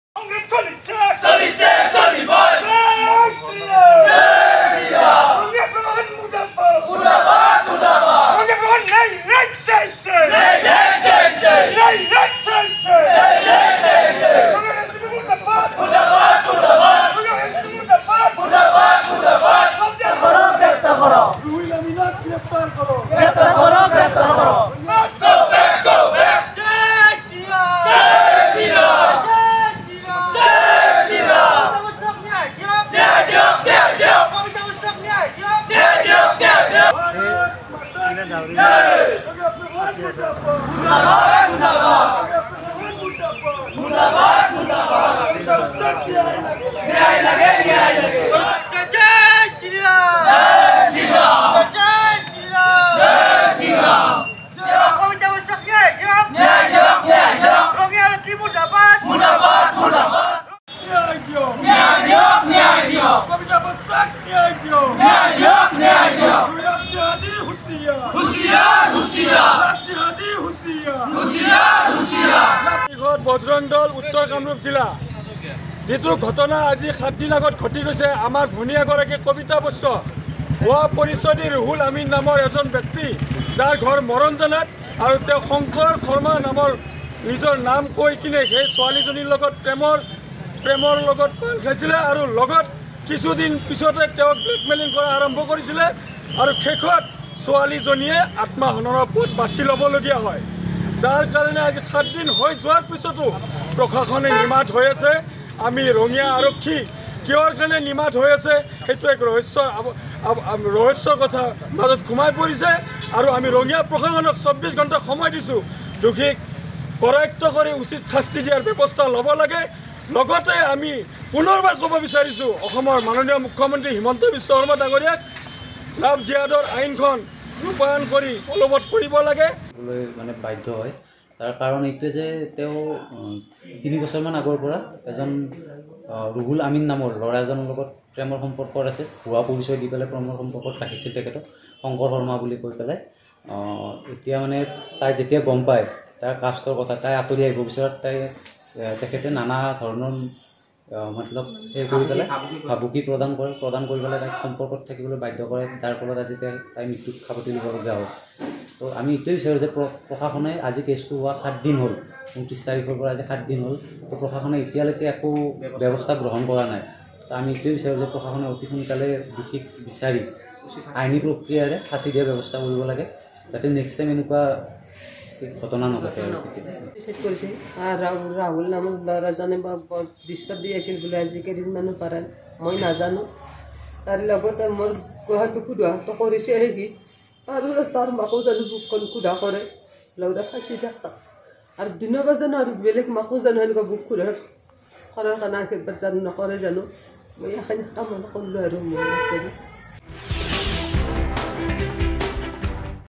এগৰাকী যুৱতীৰ মৃত্যুক লৈ ৰঙিয়াত উত্তপ্ত পৰিস্থিতি
এগৰাকী যুৱতীৰ মৃত্যুক লৈ ৰঙিয়াত উত্তপ্ত পৰিস্থিতি । পথ অৱৰোধ , থানা ঘেৰাও বজৰংগ দলৰ ।